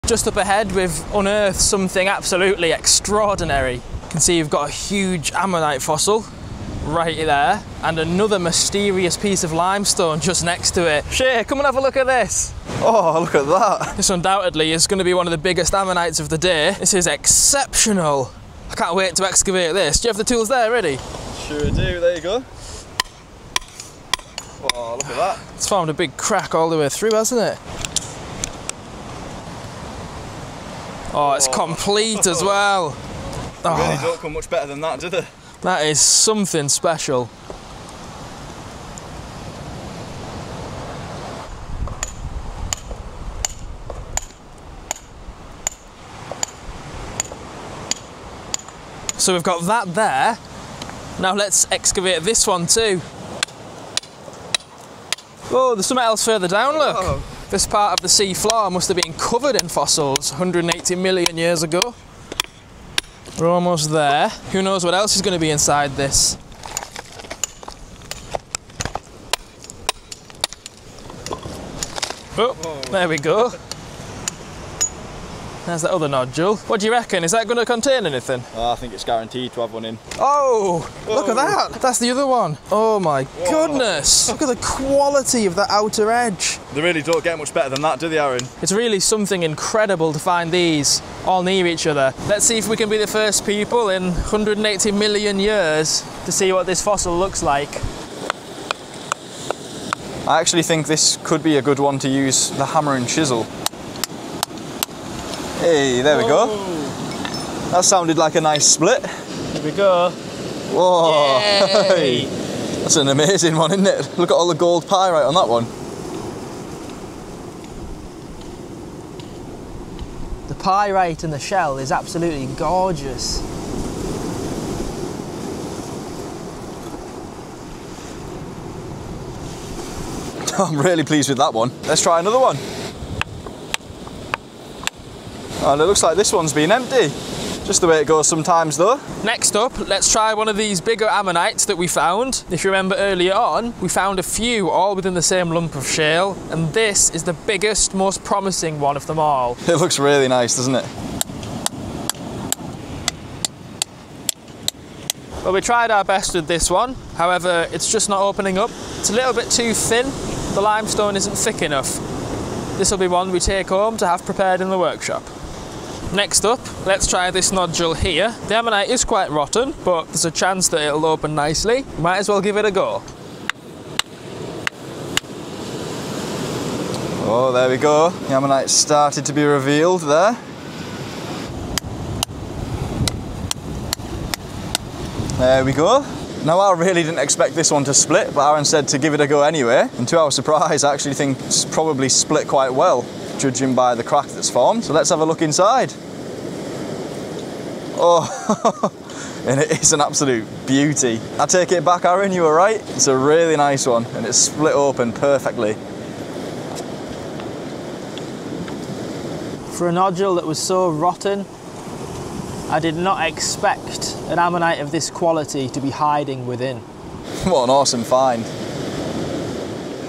We Found a Gorgeous Ammonite in a Fresh Landslide!
Here is another video from one of our fossil hunts, make sure to stick around to the end of the video to see all of the fossils we find! We have many other upcoming videos just like this episode, with some even more exciting fossil finds, plus more new content! This is an insight into the past, what life was like 180 Million Years Ago even before many of the most famous land-dwelling Dinosaurs ever existed.